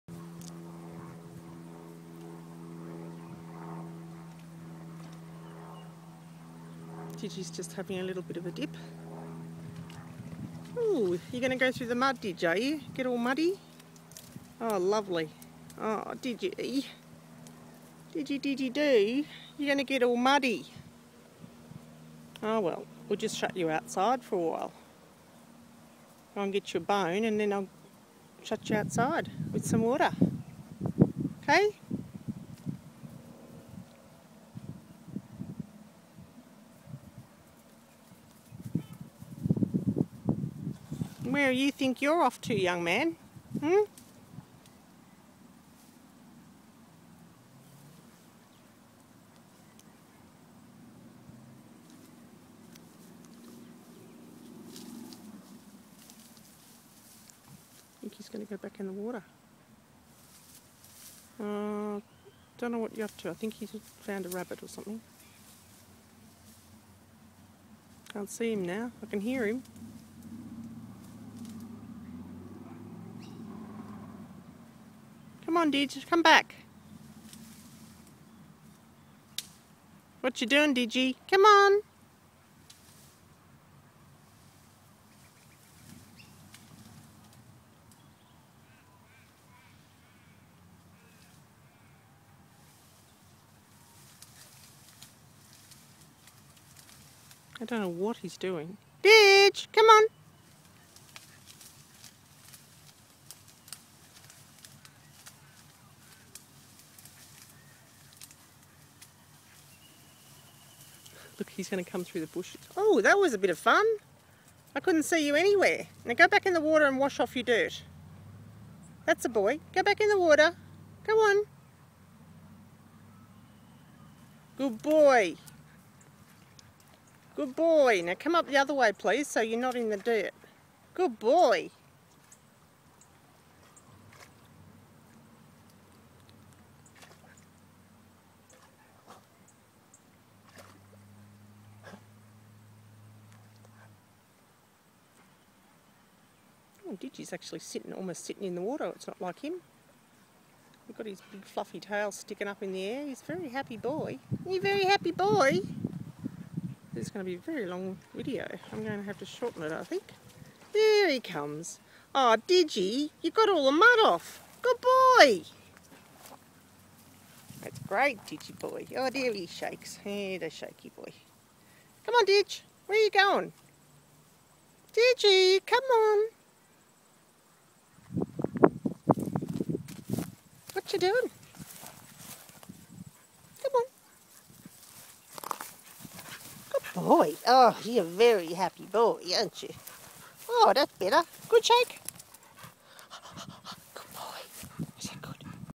Enjoying a swim & fossick in the reeds
A lovely cool down for Didge